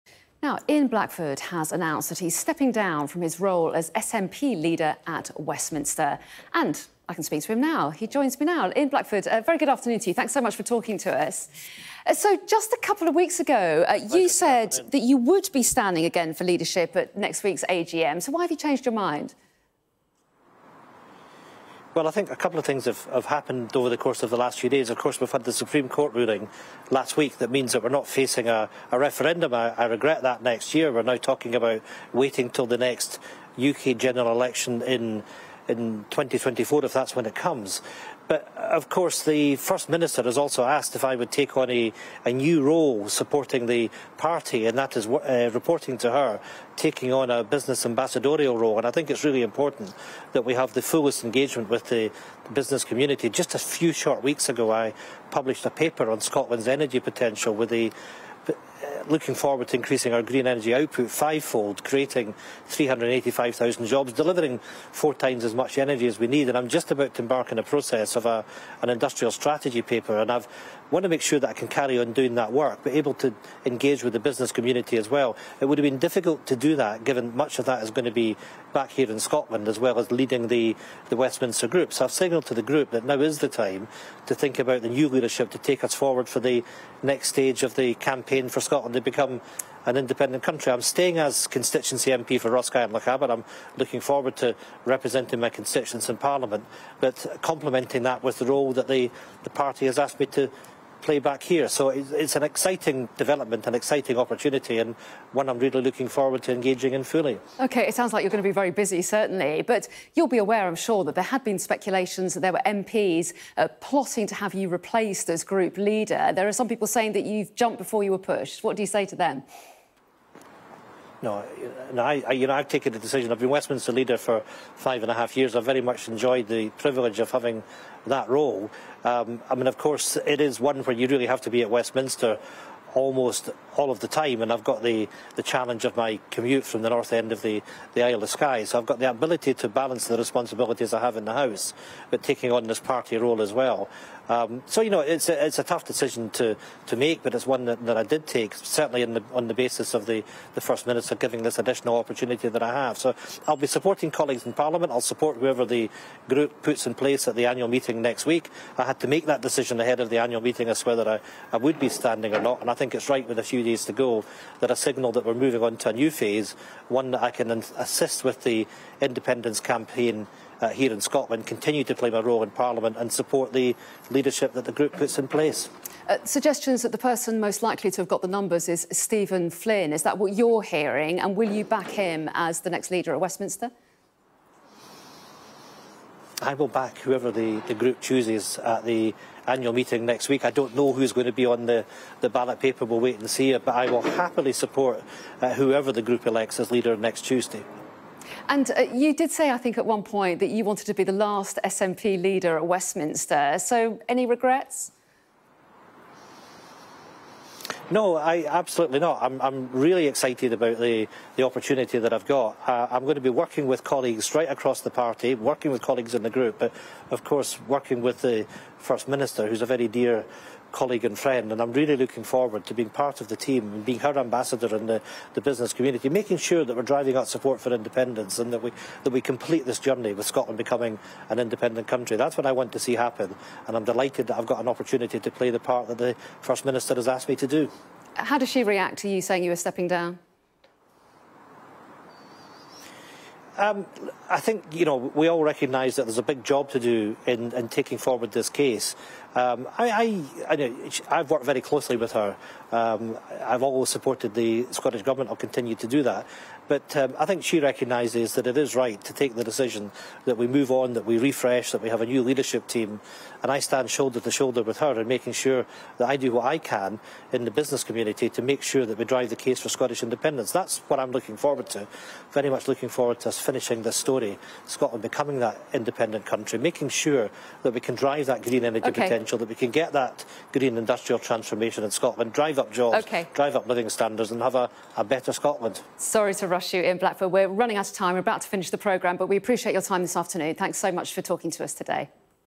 Ian Blackford: Stepping down is a 'tough decision'
Ian Blackford says stepping down from his role as Westminster leader of the SNP was a "tough decision to make".